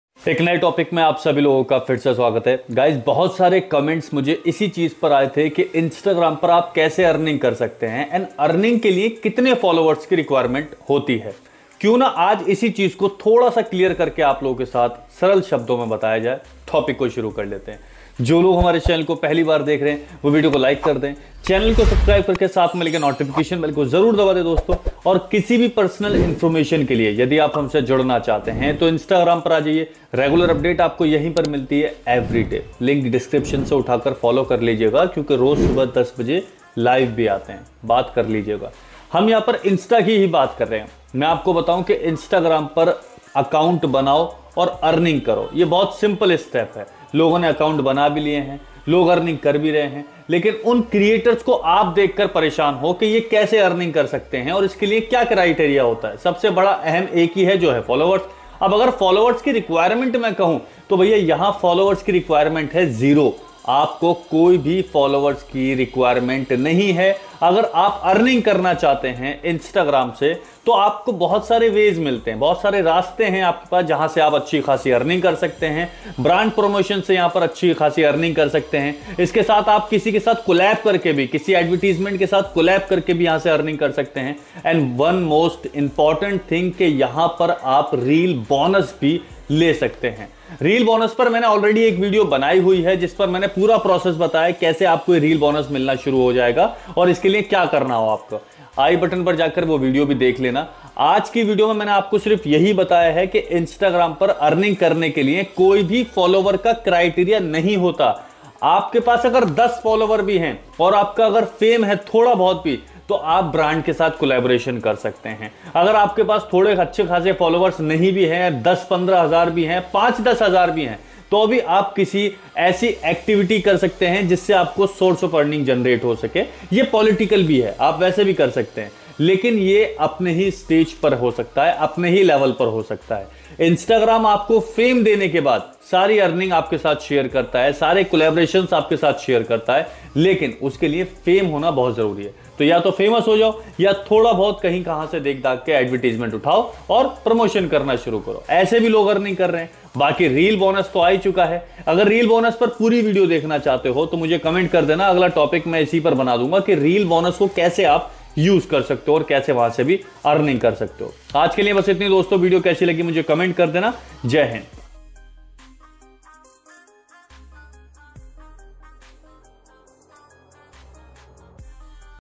Kitne Followers Par Earning Hoti Hai Instagram Se | How To Earn Money From Instagram | Instagram Tip
To Dosto Aaj Me Apko Bataunga Ki :-  Kitne Followers Par Earning Hoti Hai Instagram Se | How To Earn Money From Instagram | Instagram Earning Tips Hindi | Gott Technical 2023

( जयहिंद )